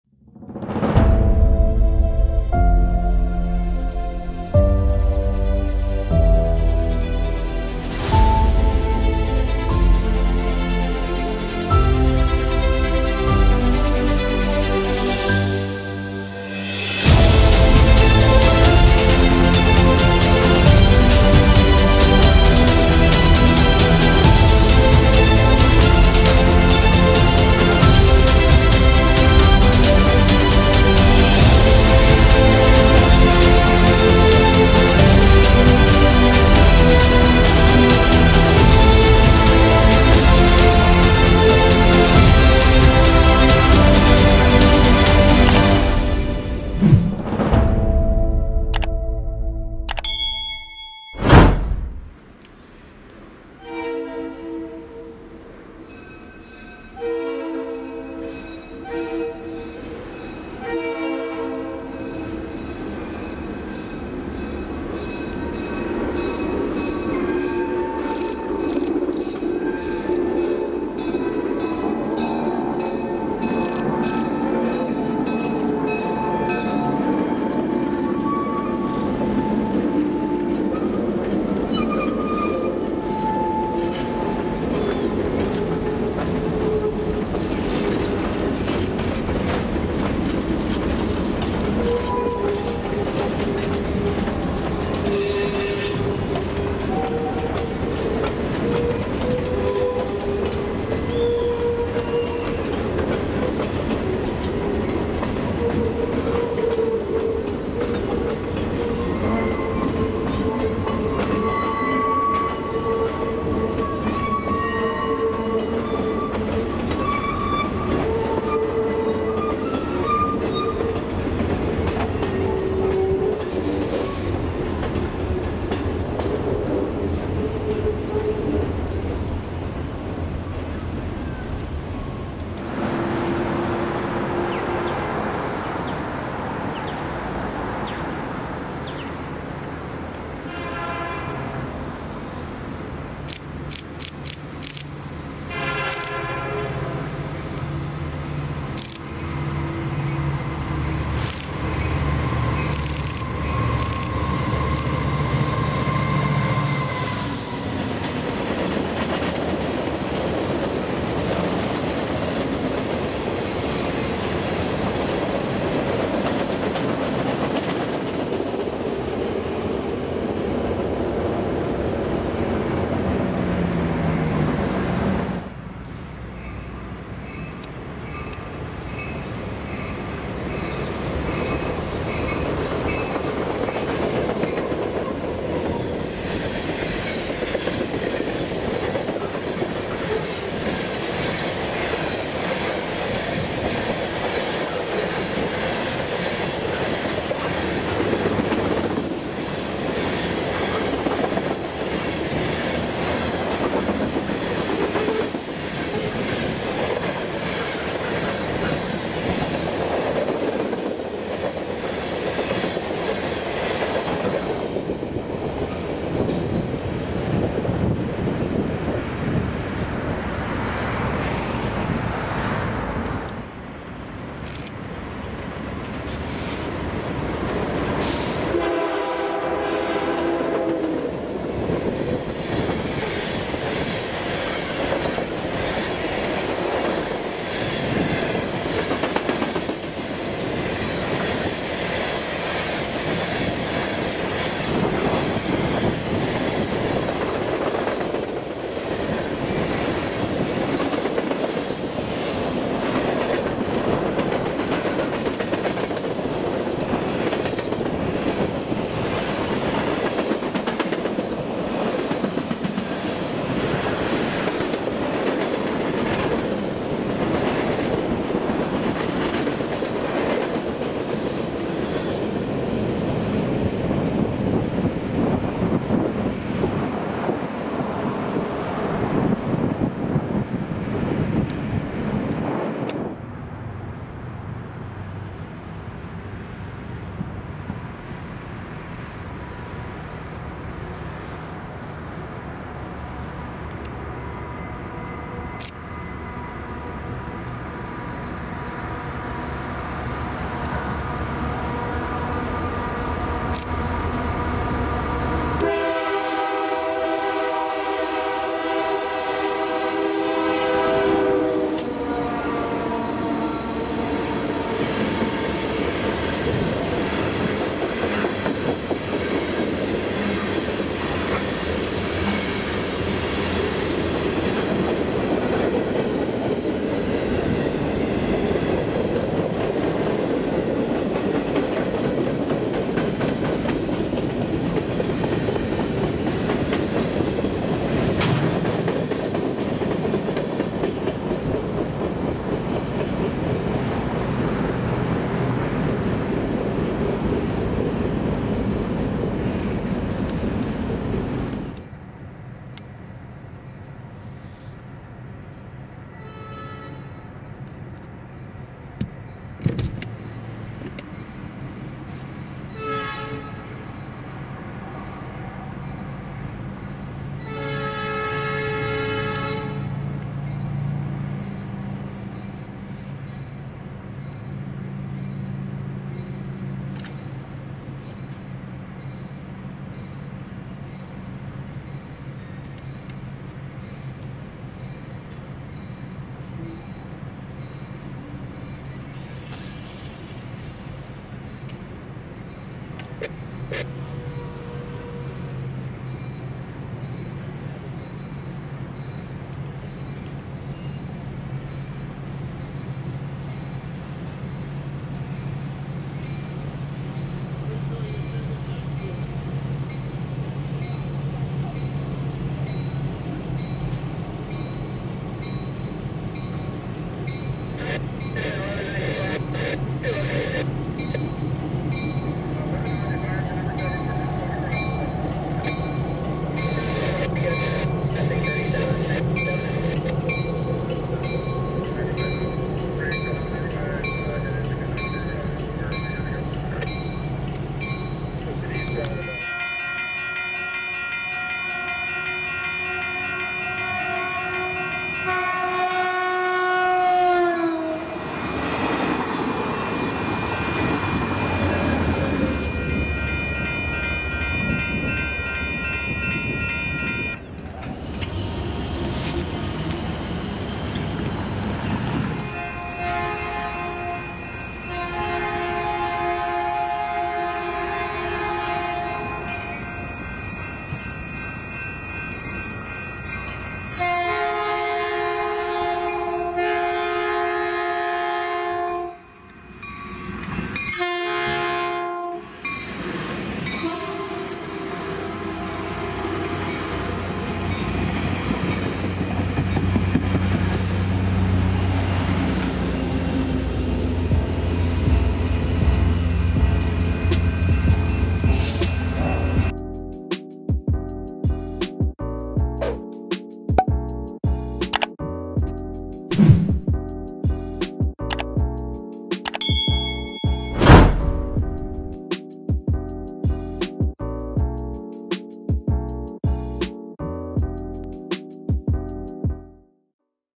[4K] A Few Days of Trains in San Jose, Roaring Camp, NCRY, and Burlingame
(Watch in 4K Quality for Best Viewing Experience)
This is a compilation of a few days worth of trains in the course of about a month or so featuring UP 1005 on an Extra Hollister Local, a friend of mine (Will not disclose his name) passing by multiple times on the Salinas Hauler, the Beach Train at Roaring Camp,  the passenger train that was taking passengers to different locations for the "Ride #7 for the #1744" Photographers Special in Pleasanton, and a short railfan session in Burlingame. Enjoy!